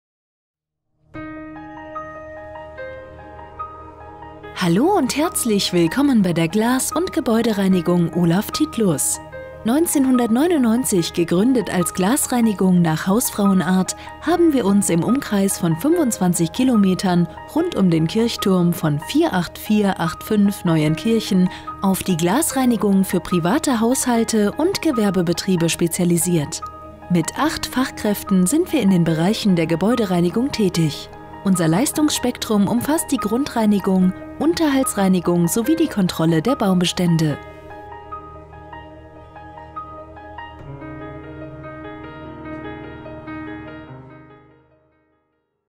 Gebäudereinigung Titlus GmbH & Co. KG in Neuenkirchen
Ihre spezialisierte Fenster- und Gebäudereinigung mit Sitz in Neuenkirchen
Mehr als nur sauber
Bei uns erhalten Sie sämtliche Reinigungsdienstleistungen aus einer Hand. Sowohl die traditionelle Glas- und Fensterreinigung als auch Unterhaltsreinigungen für Treppenhäuser und Grundreinigungen für Fußböden führen wir gerne für Sie aus. Im Umkreis von 20 Kilometer rund um Neuenkirchen sind wir Ihr kompetenter Partner für Reinigungsarbeiten aller Art. Außerdem sind wir als zertifizierte Baumkontrolleure in der Lage, die gesetzlich vorgeschriebenen Kontrollen an Bäumen im öffentlichen Raum in und um Neuenkirchen durchzuführen. Bei Fragen rund um die Gebäude- und Glasreinigung steht Ihnen das kompetente Team um Olaf Titlus gerne zur Verfügung.

Gebäudereinigung Titlus GmbH & Co. KG
Daimlerstr. 6
48485 Neuenkirchen
Geschäftsführer: Olaf Titlus
Telefax: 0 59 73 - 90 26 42